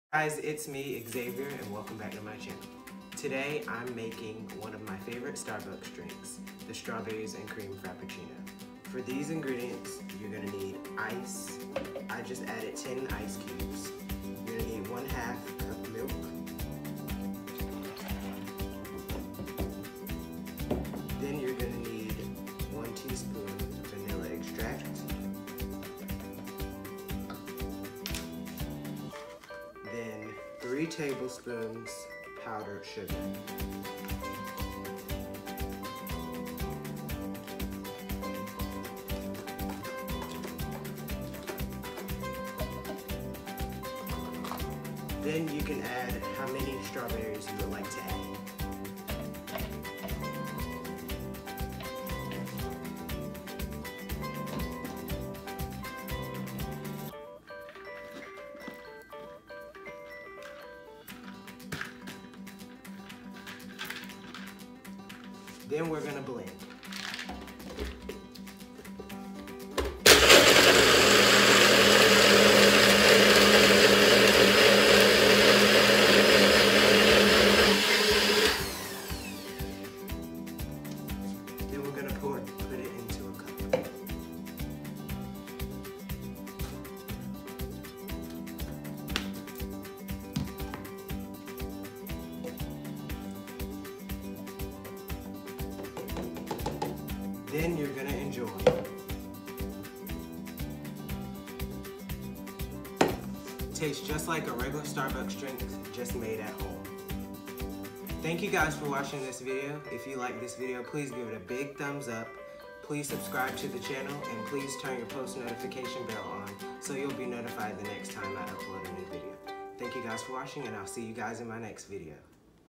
How to make a strawberries and cream frappuccino !
In today's video i'm going to be making by favorite frappuccino from starbucks. Let me know if you have any questions and put them in the comments.